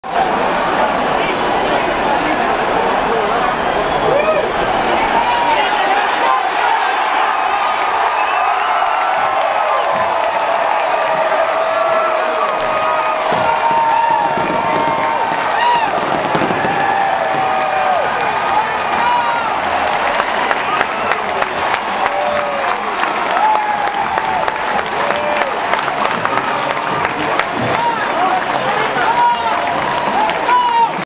Marcus Lattimore First Career Touchdown
Marcus scores his first touchdown as a gamecock from 3 yards out